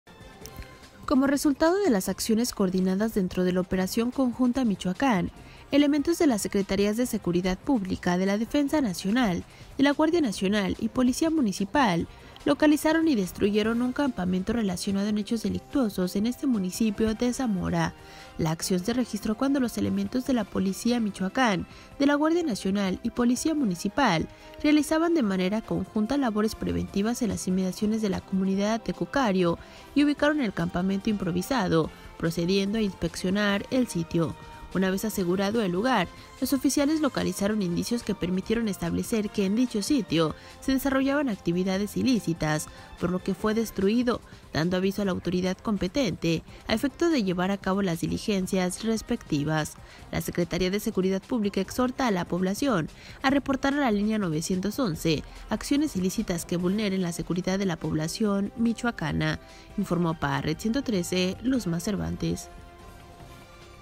Operación Conjunta Michoacán logra destrucción de narcocampamento en Zamora
Zamora, Mich.- 27 de enero de 2021.- Como resultado de las acciones coordinadas dentro de la Operación Conjunta Michoacán, elementos de las secretarías de Seguridad Pública (SSP), de la Defensa Nacional (SEDENA), de la Guardia Nacional (GN), y Policía Municipal, localizaron y destruyeron un campamento relacionado en hechos delictuosos, en este municipio de Zamora.

La acción se registró cuando los elementos de la Policía Michoacán, de la GN y Policía Municipal realizaban de manera conjunta labores preventivas en las inmediaciones de la comunidad de Atecucario, y ubicaron el campamento improvisado, procediendo a inspeccionar el sitio.

Una vez asegurado el lugar, los oficiales localizaron indicios que permitieron establecer que en dicha sitio se desarrollaban actividades ilícitas, por lo que fue destruido, dando aviso a la autoridad competente, a efecto de llevar a cabo las diligencias respectivas.

La SSP exhorta a la población a reportan a la línea  911, acciones ilícitas que vulneren seguridad de la población michoacana.